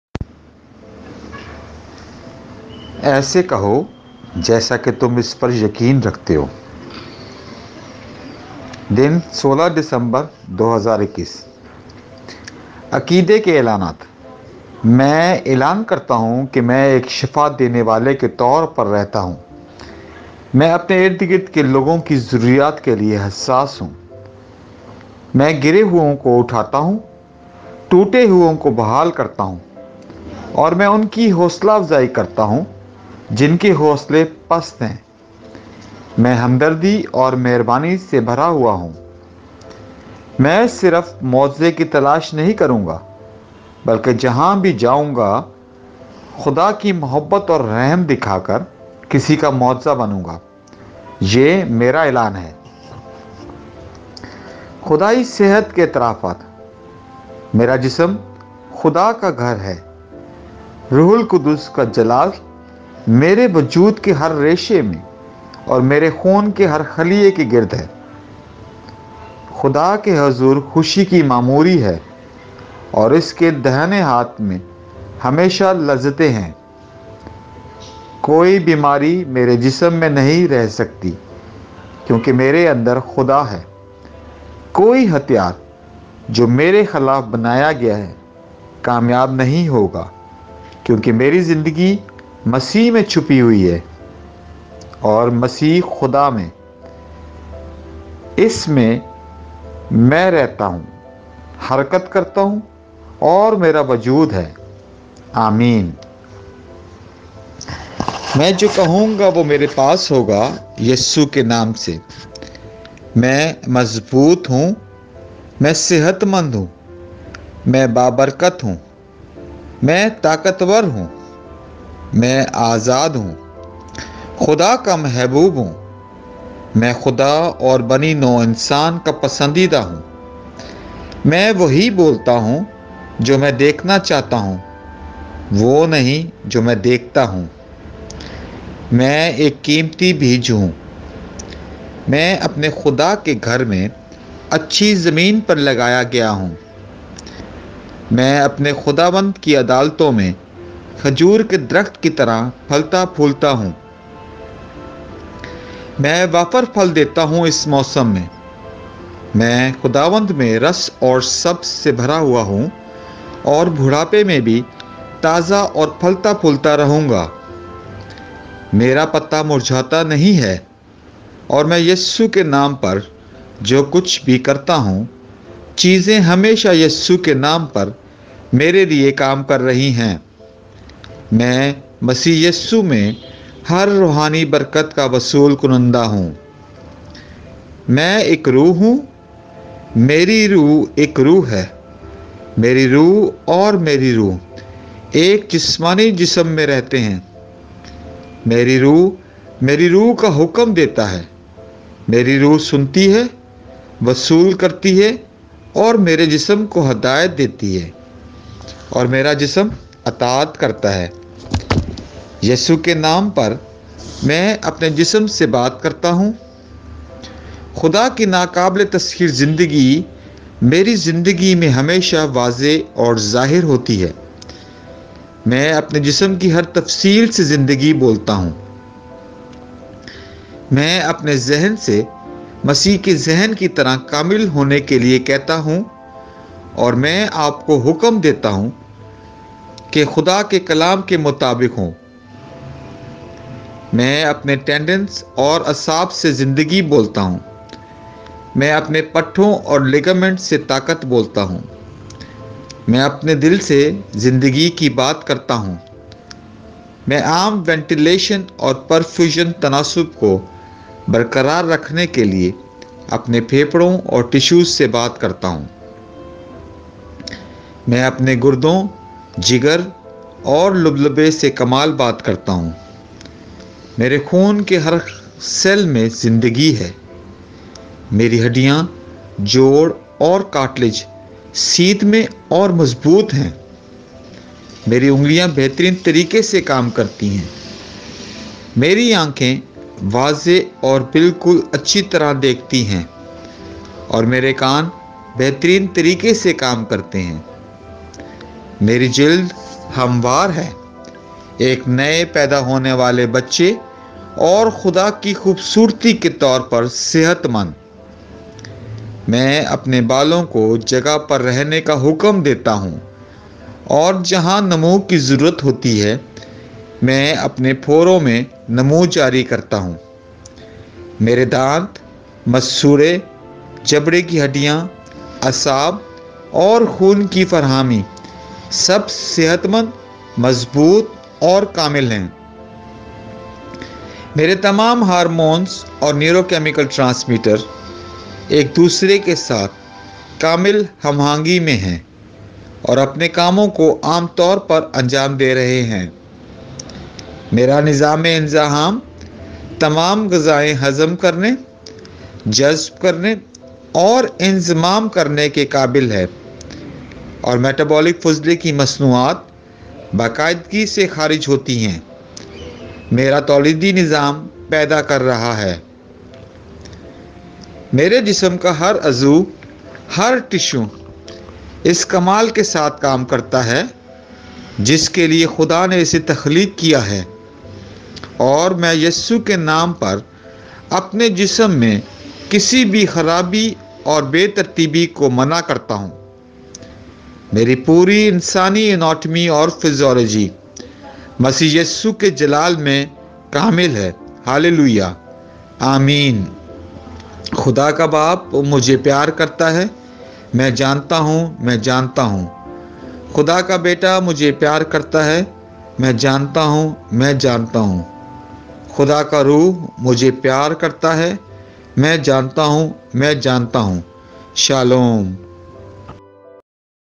16th December 2021 Say It like You Mean It (Audio in Urdu)
دسمبر  2021۔اسے کہو جیسا کہ تم اس پر یقین رکھتے ہو 16

عقیدے کے اعلانات
میں اعلان کرتا ہوں کہ میں ایک شفا دینے والے کے طور پر رہتا ہوں میں اپنے ارد گرد کے لوگوں کی ضروریات کے لیے حساس ہوں۔ میں گرے ہوئے کو اٹھاتا ہوں، ٹوٹے ہوئے کو بحال کرتا ہوں،اور میں انکی حوصلہ افزائی کرتا ہوں جنکے حوصلے پست ہیں ، میں ہمدردی اور مہربانی سے بھرا ہوا ہوں۔ میں صرف معجزے کی تلاش نہیں کروں گا، بلکہ جہاں بھی جاؤں خدا کی محبت اور رحم دکھا کر کسی کا معجزہ بنوں گا۔ یہ میرا اعلان ہے۔

خدائی صحت کے اعترافات
میرا جسم خدا کا گھر ہے۔ روح القدس کا جلال میرے وجود کے ہر ریشے اور میرے خون کے ہر خلیے کے گرد ہے۔ خُدا کے حضور خوشی کی معموری ہے اور اُس کے دہنے ہاتھ میں ہمیشہ لذتیں ہیں۔ کوئی بیماری میرے جسم میں نہیں رہ سکتی کیونکہ میرے اندر خدا ہے۔ کوئی ہتھیار جو میرے خلاف بنایا گیا ہے کامیاب نہیں ہو گا، کیونکہ میری زندگی مسیح میں چھپی ہوئی ہے، اور مسیح خدا میں۔ اس میں، میں رہتا ہوں، حرکت کرتا ہوں، اور میرا وجود ہے آمین

میں جو کہوں گا وہ میرے پاس ہوگا یسوع کے نام سے
میں مضبوط ہوں
 میں صحت مند ہوں
 میں بابرکت ہوں
میں طاقتور ہوں
 میں آزاد ہوں میں
 خدا کا محبوب ہوں
 میں خدا اور بنی نوع انسان کا پسندیدہ ہوں
میں وہی بولتا ہوں جو میں دیکھنا چاہتا ہوں وہ نہیں جو میں دیکھتا ہوں۔
 میں ایک قیمتی بیج ہوں
 میں اپنے خدا کے گھر میں اچھی زمین پر لگایا گیا ہوں
 میں اپنے خداوند کی عدالتوں میں کھجور کے درخت کی طرح پھلتا پھولتا ہوں
 میں وافر پھل دیتا ہوں اس موسم میں خداوند میں رس اور سبز سے بھرا ہوا ہوں اور بڑھاپے میں بھی میں تازہ اور پھلتا پھولتا رہوں گا
 میرا پتہ مرجھاتا نہیں ہے اور میں یسوع کے نام پر جو کچھ بھی کرتا ہوں چیزیں ہمیشہ یسوع کے نام پر میرے لئے کام کر رہی ہیں
 میں مسیح یسوع میں ہر روحانی برکت کا وصول کنندہ ہوں
میں ایک روح ہوں میری روح ایک روح ہے میری روح اور میری روح ایک جسمانی جسم میں رہتے ہیں میری روح میری روح کا حکم دیتا ہے.میری روح سنتی ہے، وصول کرتی ہے اور میرے جسم کو ہدایات دیتی ہے اور میرا جسم اطاعت کرتا ہے

یسوع کے نام پر، میں اپنے جسم سے بات کرتا ہوں
 خُدا کی ناقابل تسخیر زندگی میری زندگی میں ہمیشہ واضح اور ظاہر ہوتی ہے۔
میں اپنے جسم کی ہر تفصیل سے زندگی بولتا ہوں۔
 میں اپنے ذہن سے مسیح کے ذہن کی طرح کامل ہونے کے لیے کہتا ہوں اور میں آپ کو خدا کے کلام کے مطابق ہونے کا حکم دیتا ہوں
 میں اپنے ٹینڈنز اور اعصاب سے زندگی بولتا ہوں میں اپنے پٹھوں اور لگامنٹس سے طاقت بولتا ہوں دل میں عام وینٹی لیشن اور پرفیوژن تناسب کو برقرار رکھنے کے لئے اپنے پھیپھڑوں اور ٹشوز سے بات کرتا ہوں
 میں اپنے گردوں، جگر اور لبلبے سے کمال بات کرتا ہوں
میرے خون کے ہر سیل میں زندگی ہے
 میری ہڈیاں، جوڑ اور کارٹیلیج سیدھ میں ہیں، مضبوط اور  میری انگلیاں بہترین طریقے سے کام کرتی ہیں میری آنکھیں واضح اور بالکل اچھی طرح دیکھتی ہیں، اور میرے کان بہترین طریقے سے کام کرتے ہیں
 میری جلد ہموار ہے،  ایک نئے پیدا ہونے والے بچے اور خدا کی خوبصورتی کے طور پر صحت مند میں اپنے بالوں کو جگہ پر رہنے کا حکم دیتا ہوں اور جہاں نمو کی ضرورت ہوتی ہے میں اپنے پھوڑوں میں نمو جاری کرتا
میرے دانت ، مسوڑھے ، جبڑے کی ہڈیاں اور اعصاب اور خون کی فراہمی سب صحت مند ، مضبوط اور کامل ہیں
میرے تمام ہارمونز اور نیورو کیمیکل ٹرانسمیٹر ایک دوسرے کے ساتھ کامل ہم آہنگی میں ہیں اور اپنے کاموں کو عام طور پر انجام دے رہے ہیں۔
میرا نظام انہضام تمام غذائیں ہضم کرنے ، جذب کرنے اور انضمام کرنے کے قابل ہے اور میٹابولک فضلے کی مصنوعات باقاعدگی سے خارج ہوتی ہیں۔ میرا تولیدی نظام پیدا کر رہا ہے
میرے جسم کا ہر عضو، ہر ٹشو کمال کے ساتھ کام کرتا ہے جس کے لیے خدا نے اسے تخلیق کیا ہے، اور میں یسوع کے نام پر اپنے جسم میں کسی بھی خرابی اور خرابی کو منع کرتا ہوں۔
میری پوری انسانی اناٹومی اور فزیالوجی مسیح یسوع کے جلال میں کامل ہے! ہیلیلویاہ! آمین

خدا کا باپ ، مجھے پیار کرتا ہے ، میں جانتا ہوں میں جانتا ہوں
خدا کا بیٹا ، مجھے پیار کرتا ہے ، میں جانتا ہوں میں جانتا ہوں۔
خدا روح مجھے پیار کرتا ہے ، میں جانتا ہوں میں جانتا ہوں۔

شالوم